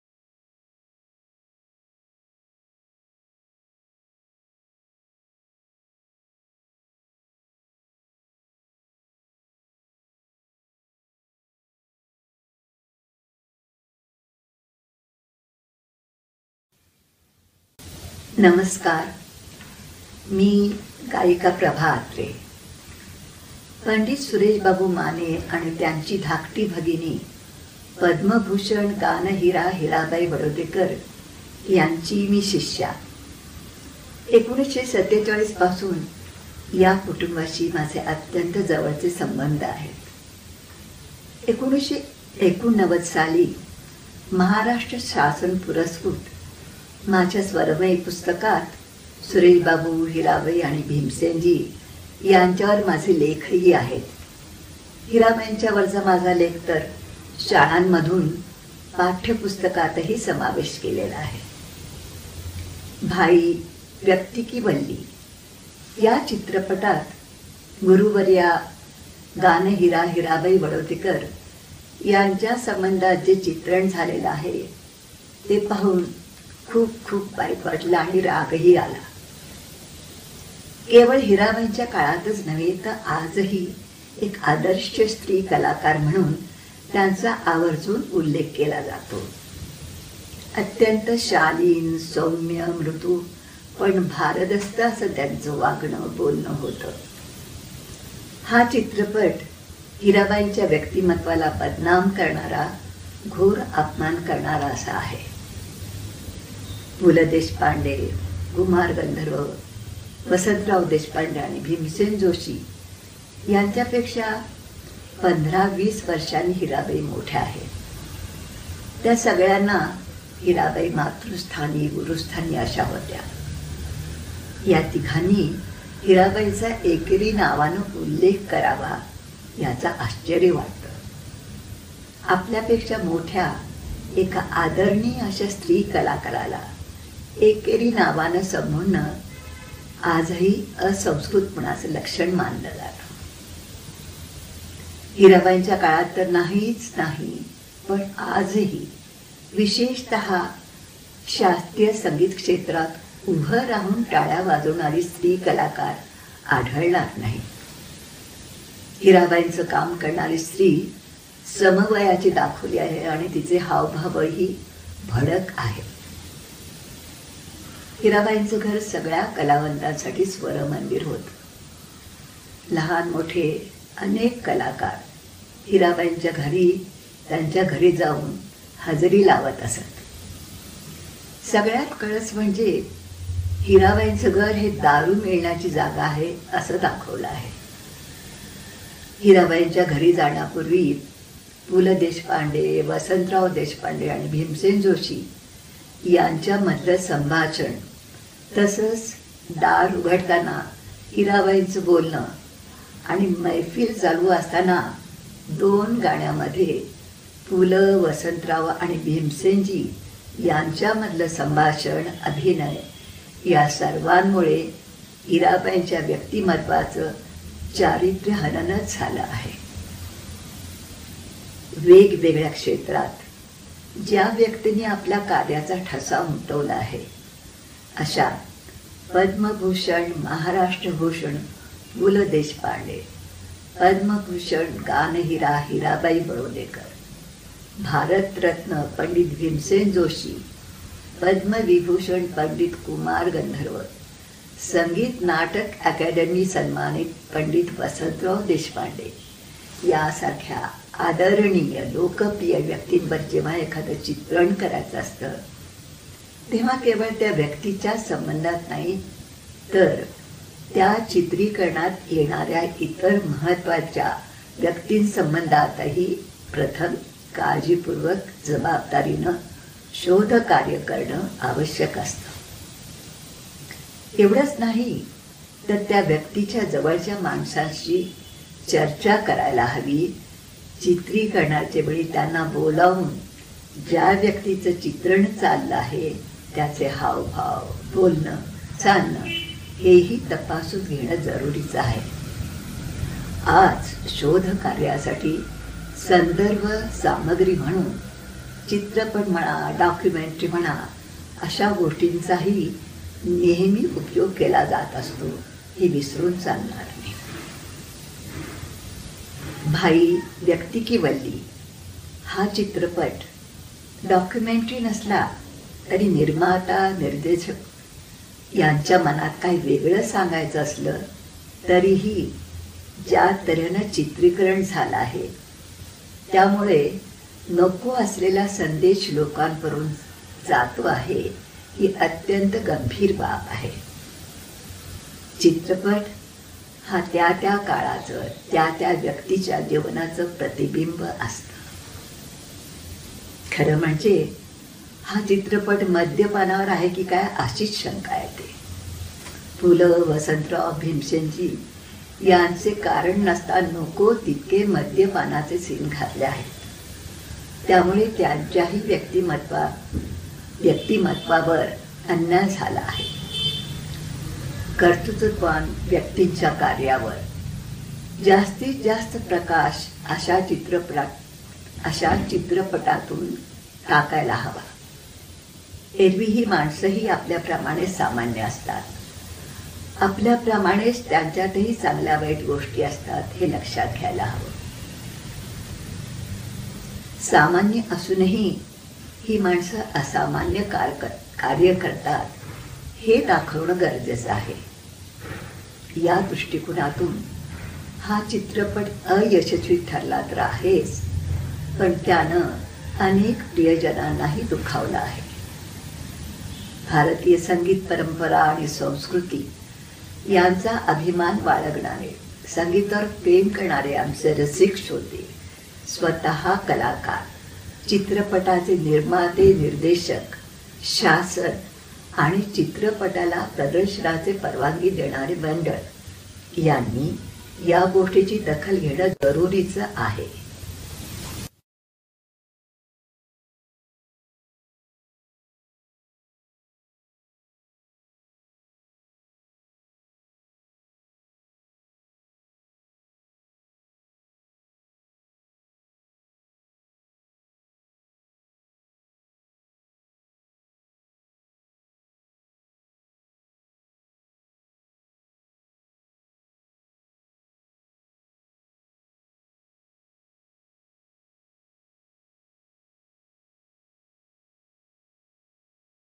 Dr. Prabha Atre protests against the movie 'Bhai : Vyakti ki Valli'
Dr. Prabha Atre protests against Bhai movie...  decries guru Hirabai Badodekar's portrayal in Pu.La. Deshpande biopic